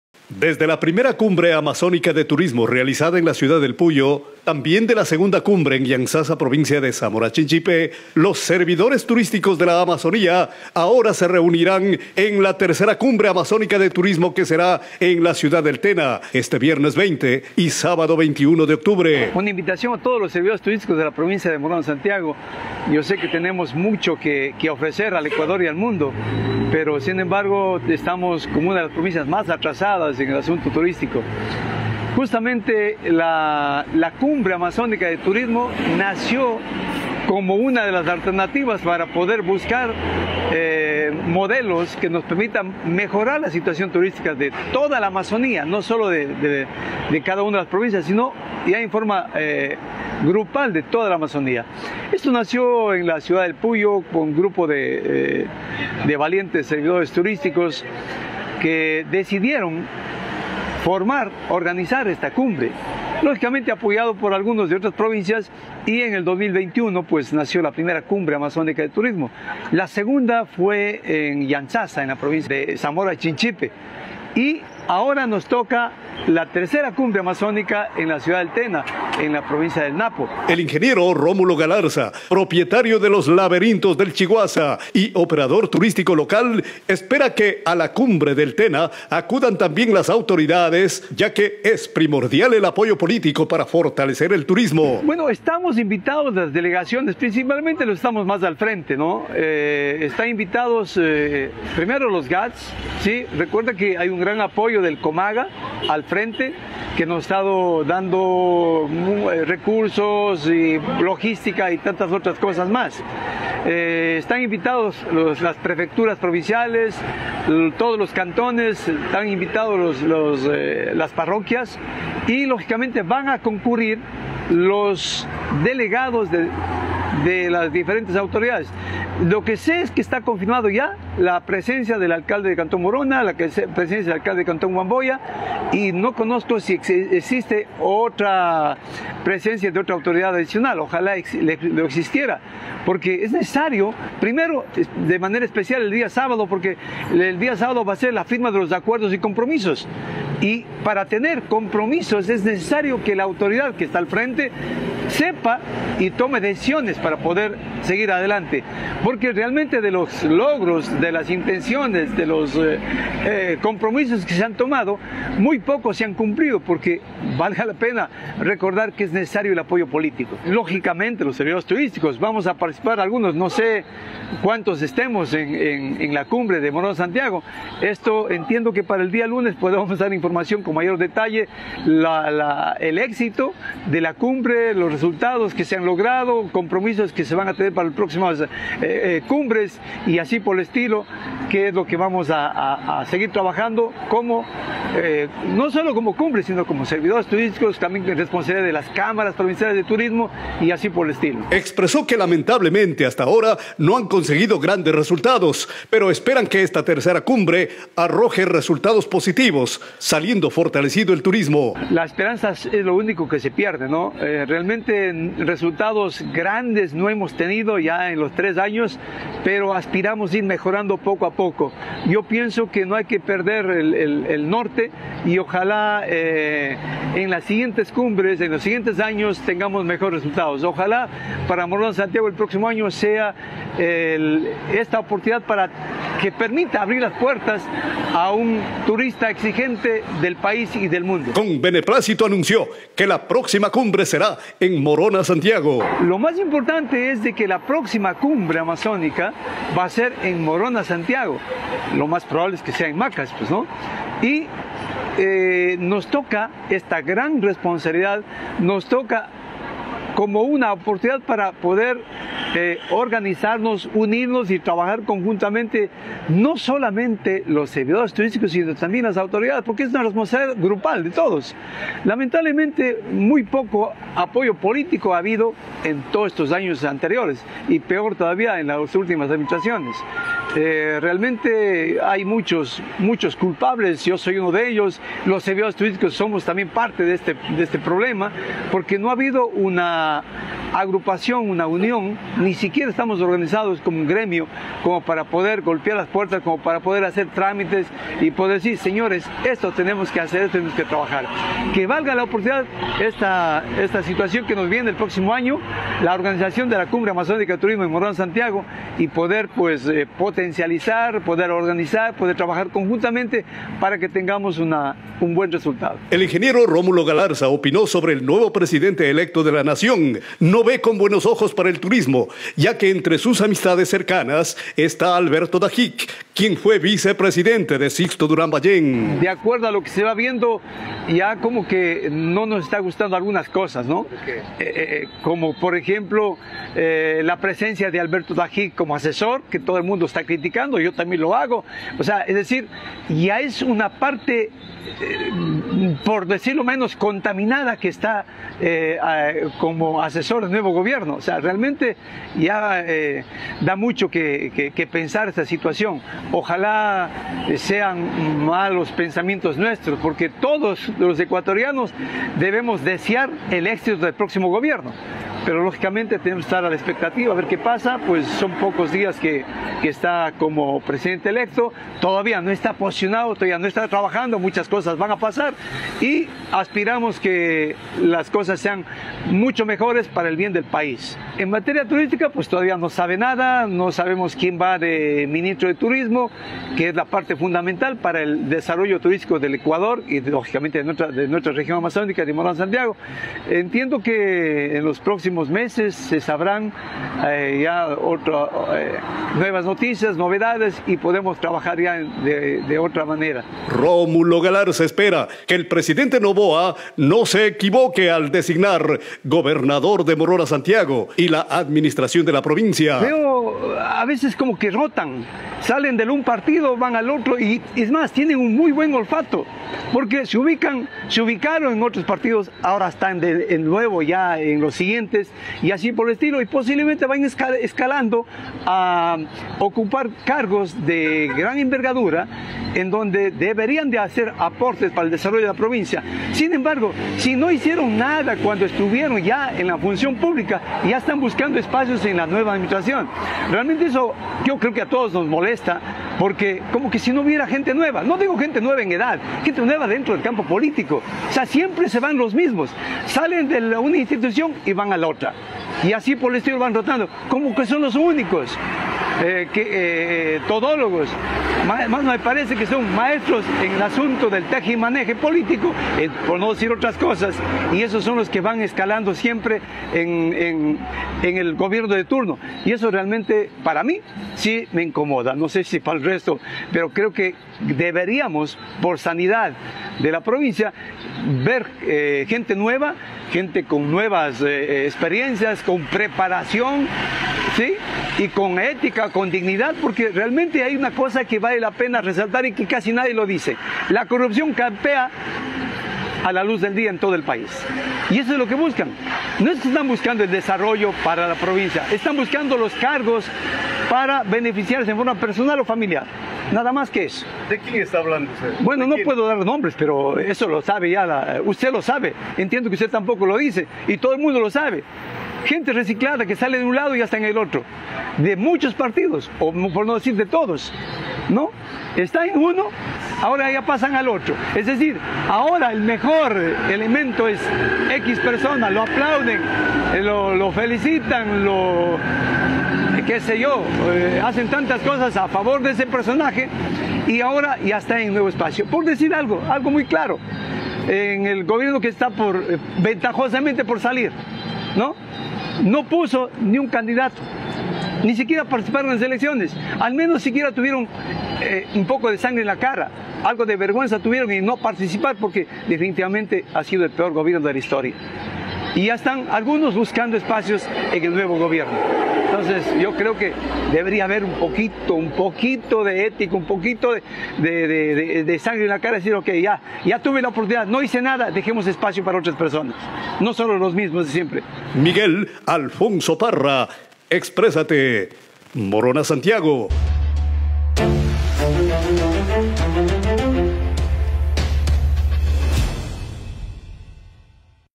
"III Cumbre Amazónica de Turismo" se desarrolla el 20 y 21 de octubre
[EXPRESIONES] EVENTOS: la "III Cumbre Amazónica de Turismo" se desarrollará este viernes 20 y sábado 21 de octubre en la ciudad del Tena, capital de la provincia del Napo.

El Ing. Rómulo Galarza a nombre de los operadores turísticos de MoronaSantiago, aspira que varias autoridades asistan al evento para impulsar la industria sin chimeneas, que requiere de un compromiso político para despuntar en forma definitiva.
De paso el entrevistado espera que el nuevo gobierno de Daniel Noboa, no se equivoque al entregar la gobernación y la administración de la provincia a las mismos rostros conocidos de la política local.